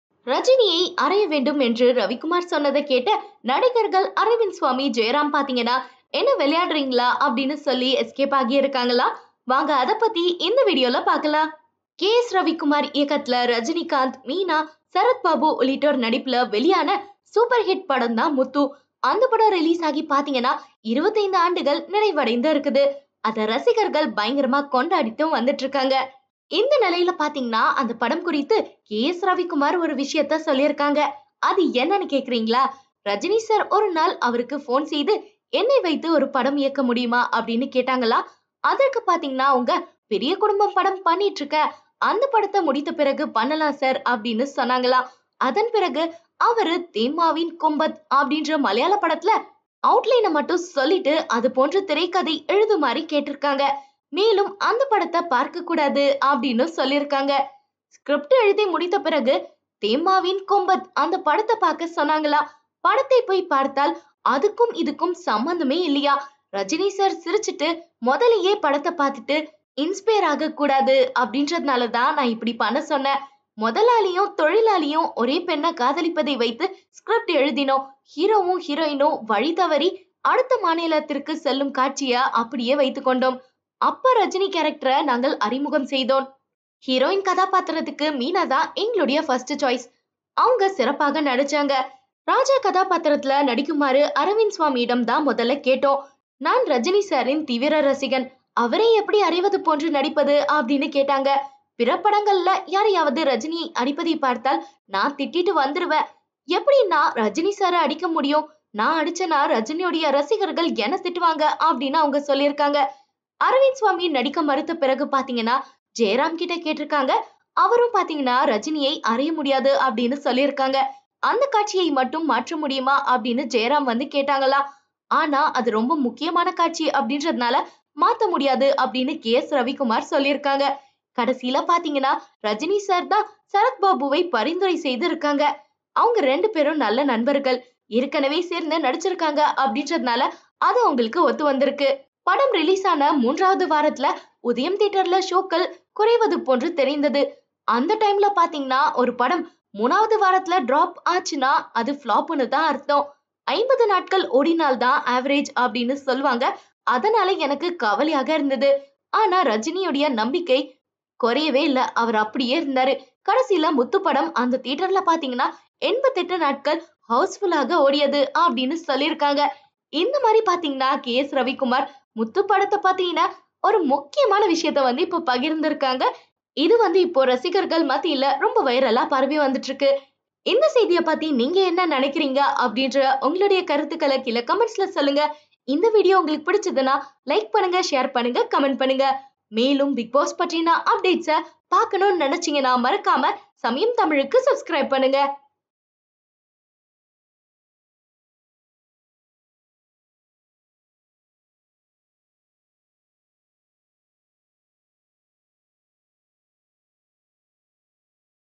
25 years of Muthu - ரஜினியை அடிக்க மறுத்த அரவிந்த்சாமி, ஜெயராம் | Samayam Tamil| Rajinikanth
ரஜினியை அறைய வேண்டும் என்று ரவிக்குமார் கூறியதை கேட்ட நடிகர்கள் அரவிந்த்சாமி, ஜெயராம் ஆகியோர் என்ன விளையாடுகிறீர்களா என்று கூறி எஸ்கேப் ஆகிவிட்டார்களாம்.